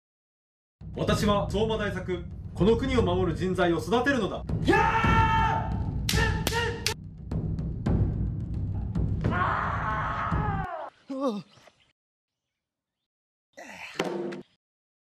16二戸市（にのへし）「歴史めざめるまち、にのへ」第20回IATふるさとCM大賞
【20回目を迎えたふるさとCM大賞】
岩手県内の市町村自らがCMを制作し、
魅力を発信する「ふるさとCM大賞」。
20回の節目で「IATふるさとCM大賞」と装いも新たにお送りします。
15秒に込めた市町村のみなさんのメッセージ！
地域を見つめ、素晴らしさを発見し、思いのこもったCMをぜひお楽しみください！